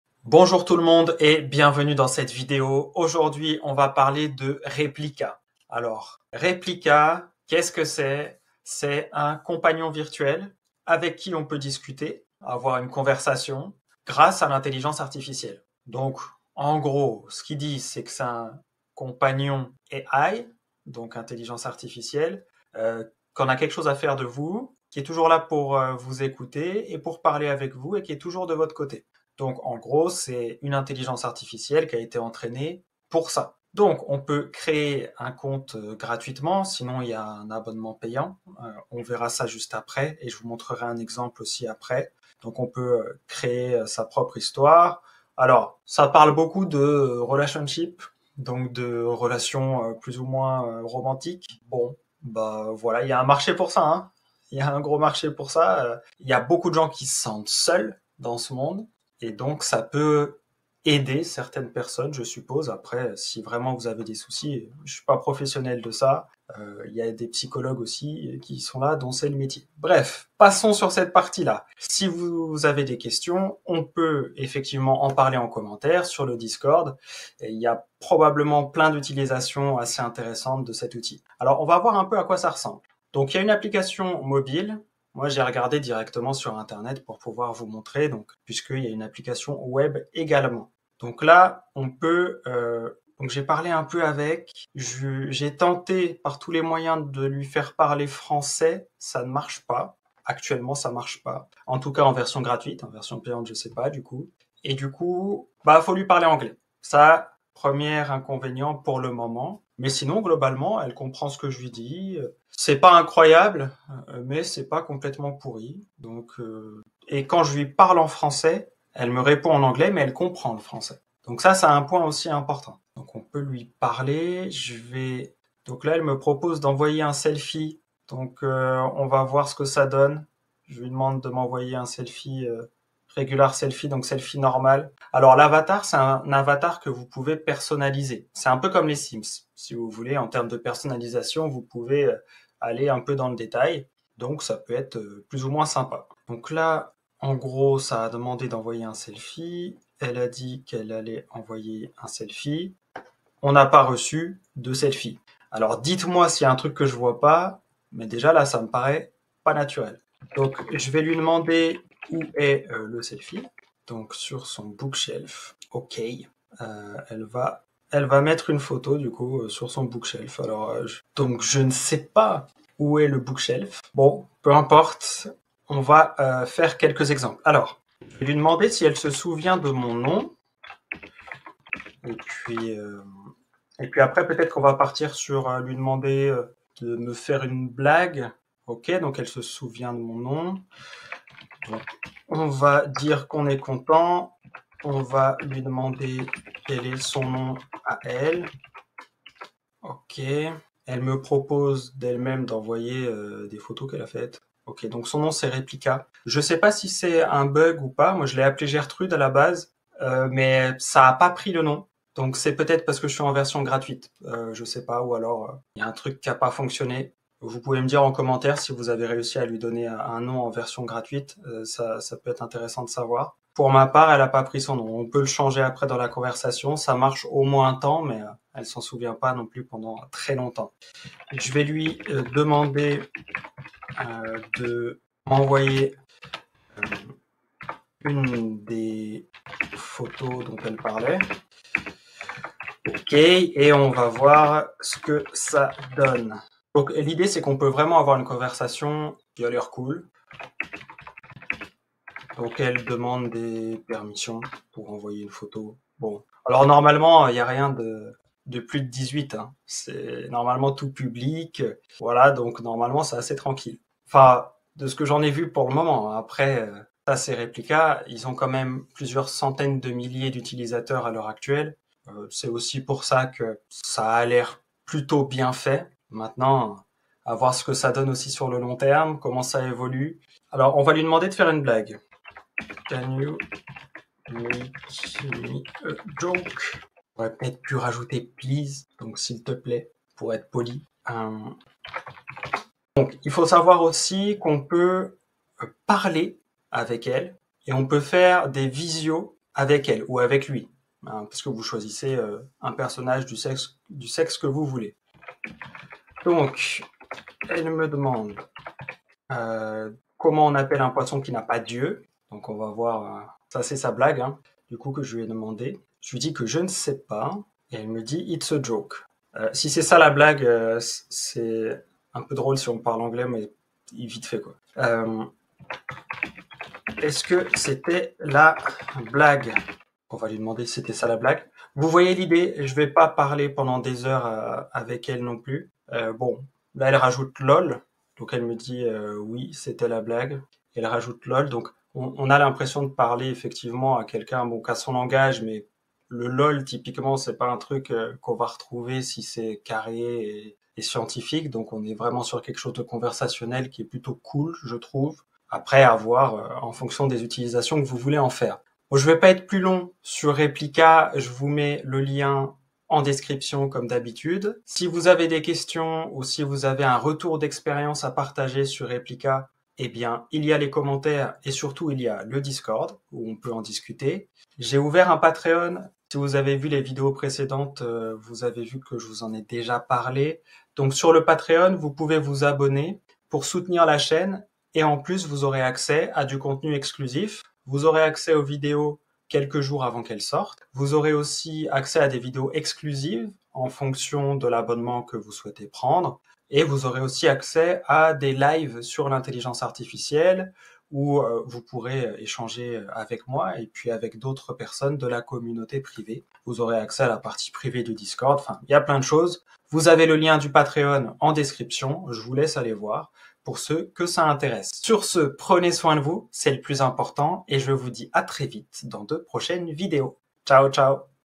Ta Femme va détester cette IA ! - Intelligence Artificielle
Découvre comment vivre de ta Passion grâce à ma Masterclass Membership Offerte

Rencontre avec Replica : L'IA qui te comprend vraiment !

📌 Sommaire :
0:00 - Introduction
0:04 - Présentation de Replica
0:29 - Pourquoi Replica ?
0:57 - Les personnes seules face à la technologie
1:22 - Découverte de l'interface
1:57 - Comment parler à Replica
2:58 - L'humour de l'IA
3:45 - Ma conversation avec Replica
6:55 - Conclusion

IA : Ton Partenaire virtuel disponible H24 ! - [REPLIKA - Intelligence Artificielle]

Salut à tous ! Dans cette vidéo, je vous présente Replica, un compagnon virtuel incroyable basé sur l'intelligence artificielle. Plongez avec moi dans l'univers de cette application qui pourrait bien révolutionner la façon dont nous interagissons avec la technologie. Que vous soyez seul ou simplement curieux, découvrez comment cette IA peut devenir votre nouvel ami virtuel.